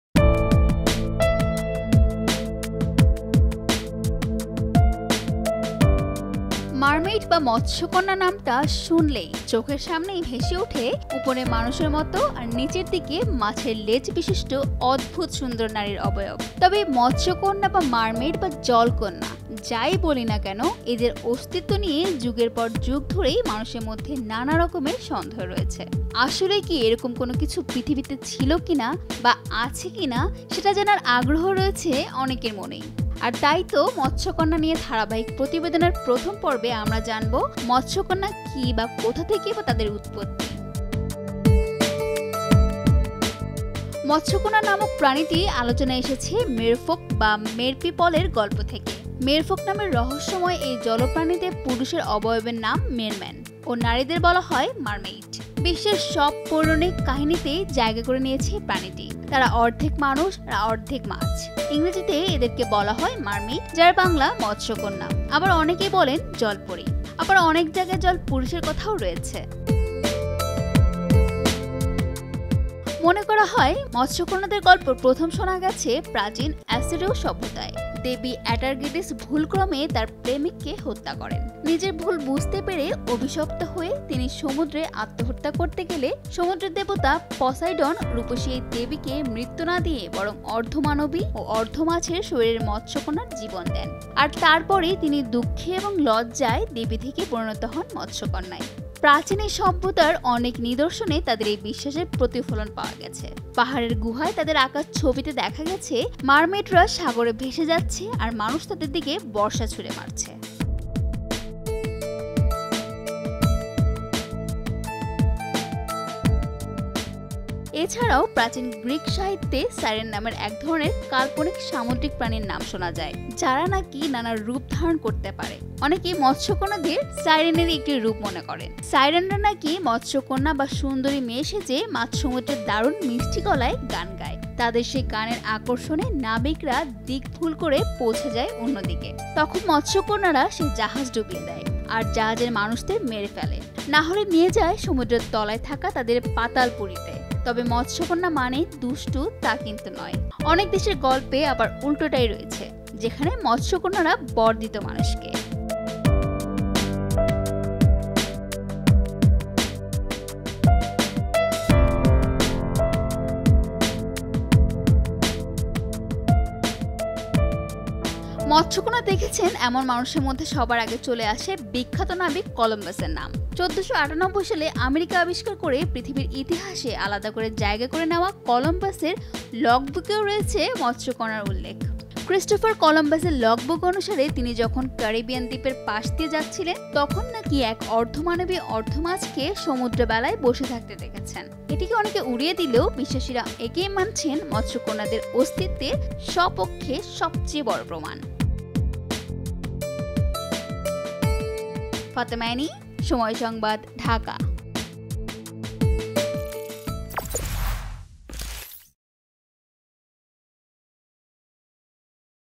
মৎস্যকন্যা; কী বা কোথায় থেকেই বা তাদের উতপত্তি? | Mermaid | Somoy TV | Somoy Entertainment
মৎস্যকন্যা নামক প্রাণীটি আলোচনায় এসেছে মেরফোক বা মের্পিপলের গল্প থেকে। মেরফোক নামের রহস্যময় এই জলজপ্রাণীদের পুরুষ অবয়বের নাম মেরম্যান ও নারীদের বলা হয় মারমেইড। বিশ্বের সব পৌরাণিক কাহিনিতেই জায়গা করে নিয়েছে এই প্রাণীটি।  তারা অর্ধেক মানুষ, আর অর্ধেক মাছ। ইংরেজিতে এদেরকে বলা হয় মারমেইড, যার বাংলা নাম মৎস্যকন্যা, আবার অনেকেই বলেন জল পরী। আবার অনেক জায়গায় জল পুরুষের কথাও রয়েছে।